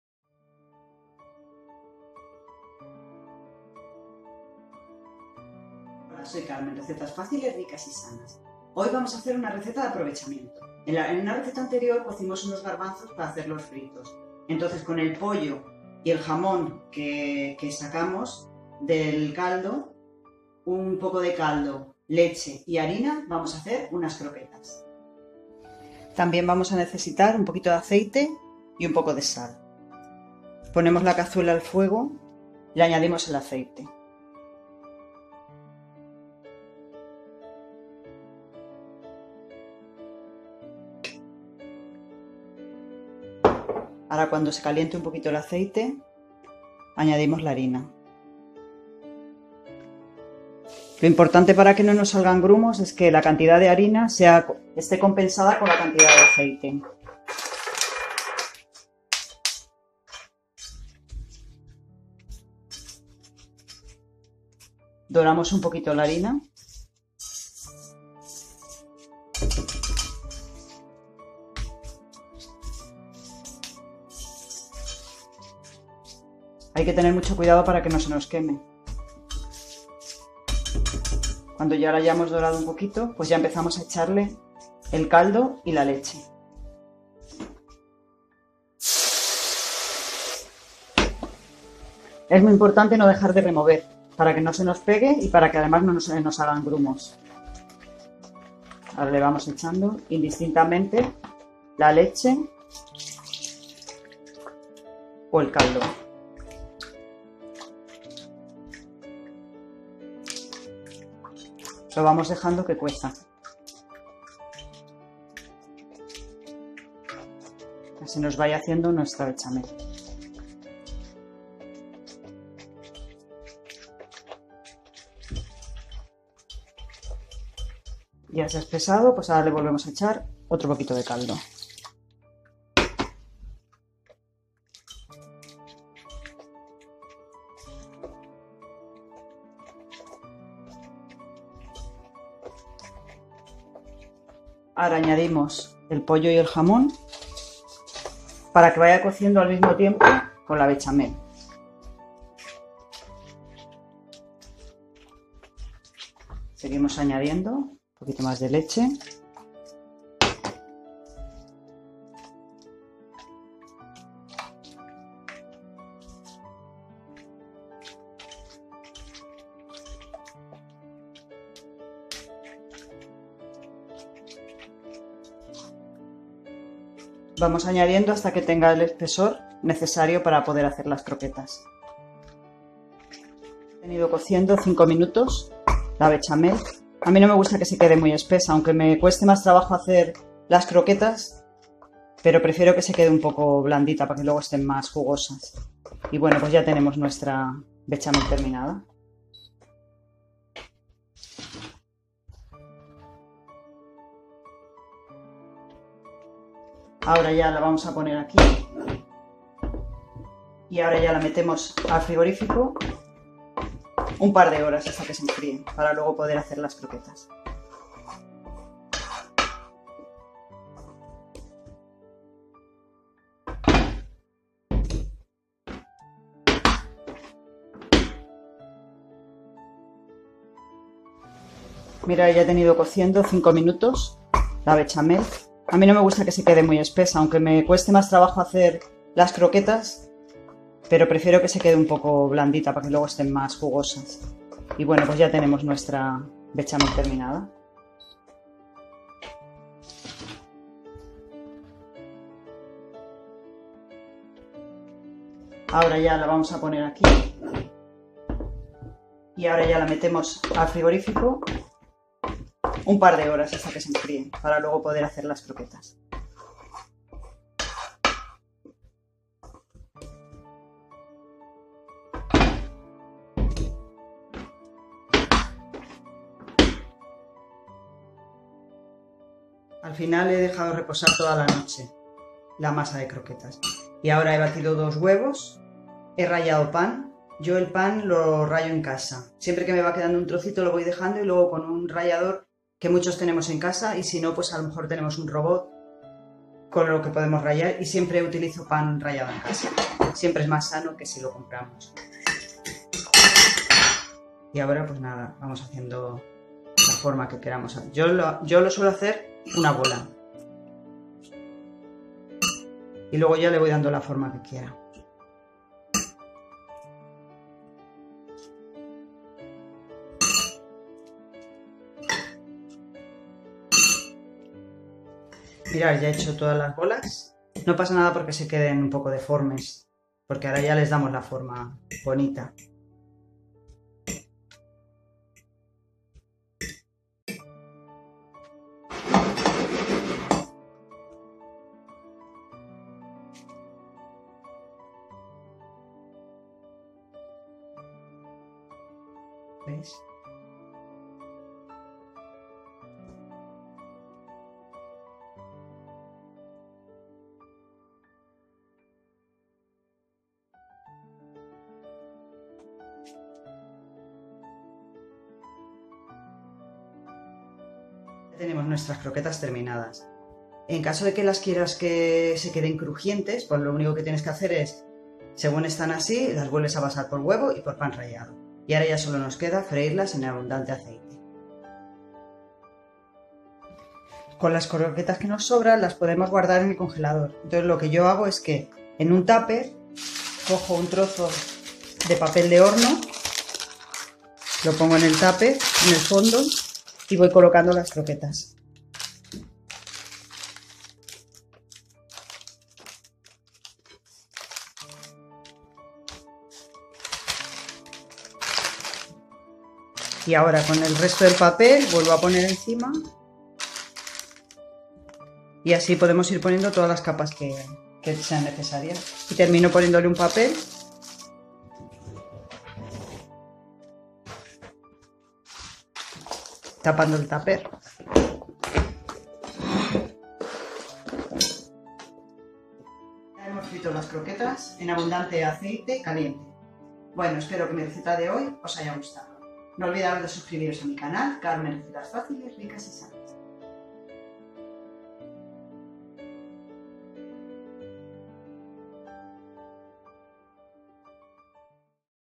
CROQUETAS DE POLLO Y JAMON
Recetas faciles, ricas y sanas. Cocina de aprovechamiento, prepara estas deliciosas croquetas de pollo y jamon.

ingredientes:
50 gr de harina
40 gr de aceite de oliva
200 gr de leche
200 gr de caldo de pollo
pollo picado
jamon picado
sal